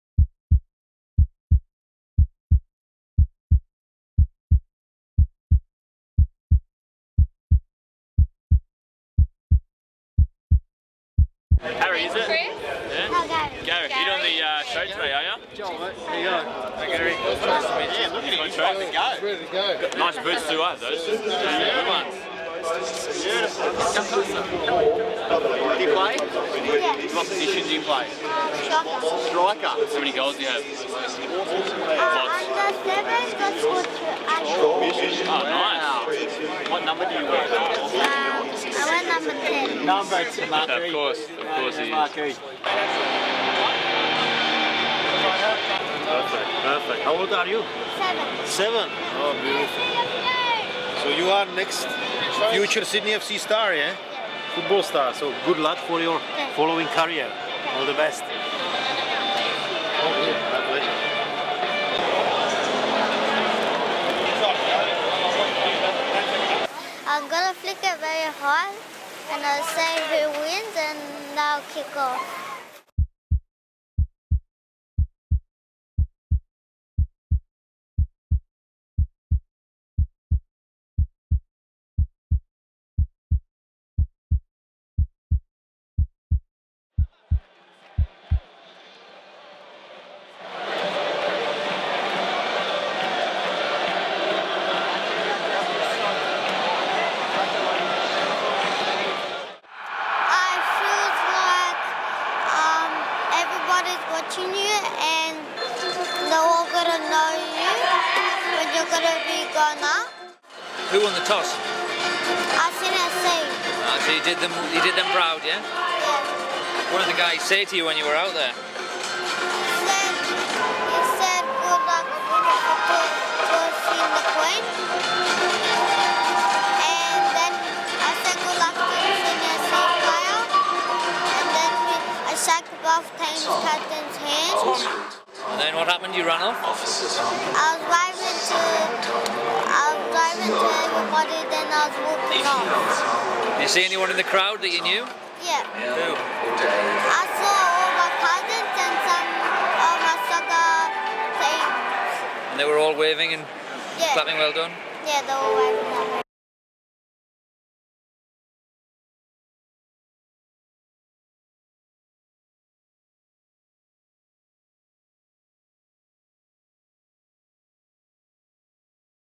Gary's Special Day
Saturday November the 19th is a day that will live long in the memory of young Sydney FC fan Gary Pavlis.